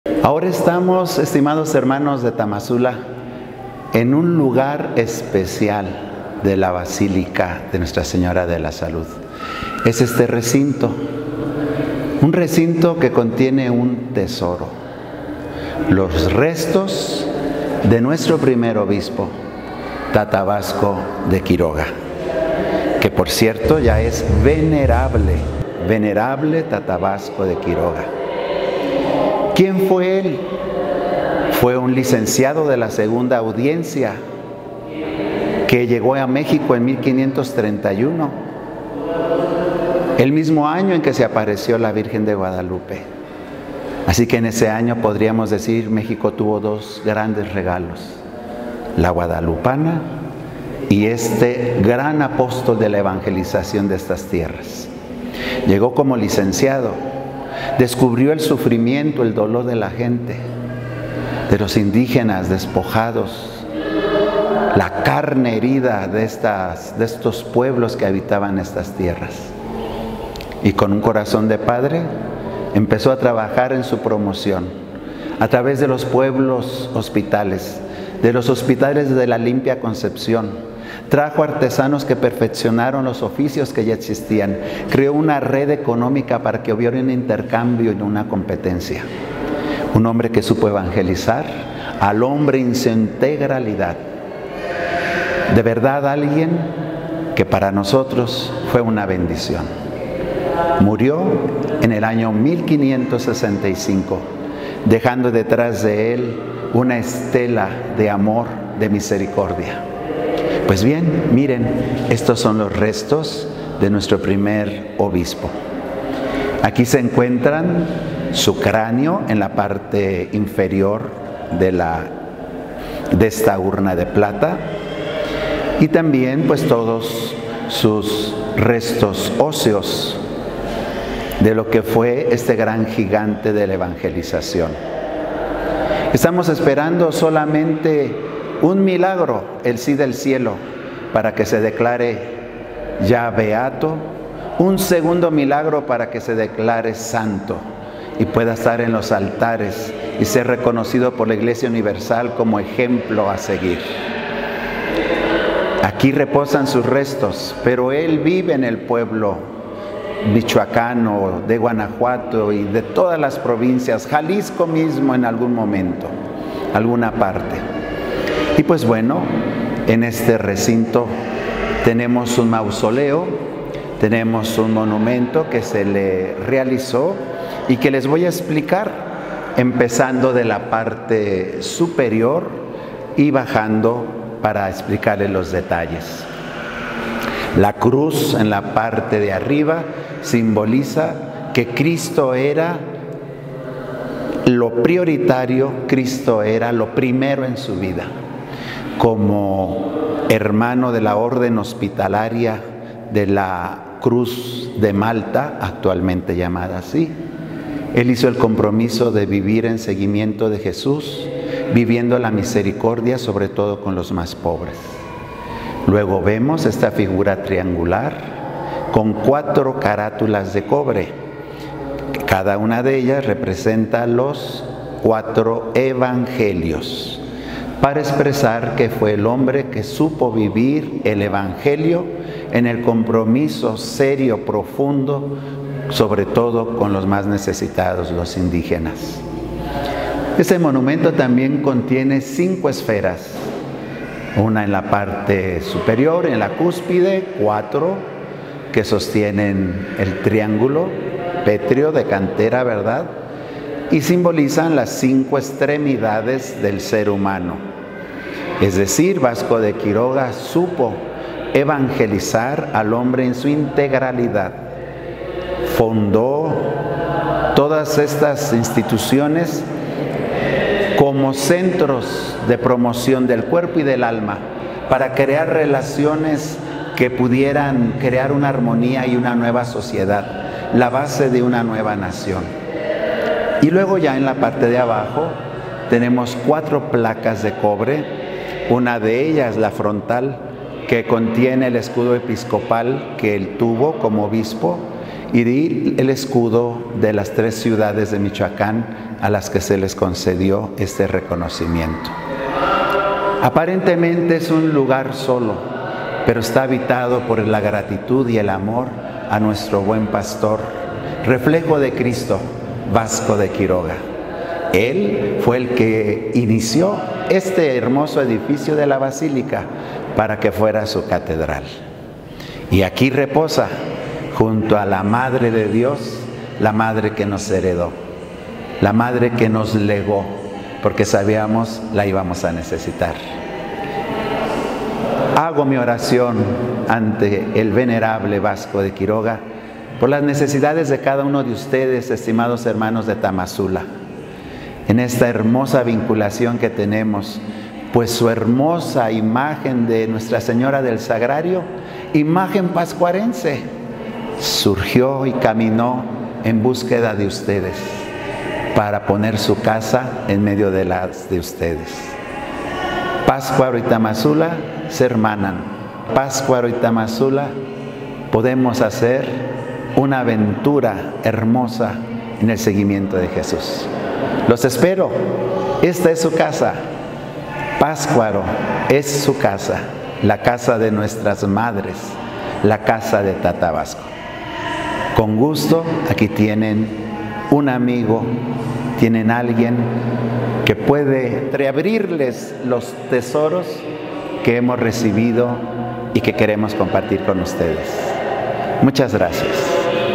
Mausoleo del Venerable Vasco de Quiroga en Basílica de Ntra. Sra. de la Salud
Vasco de Quiroga (Antigua capilla de ánimas) mausoleo de Tata Vasco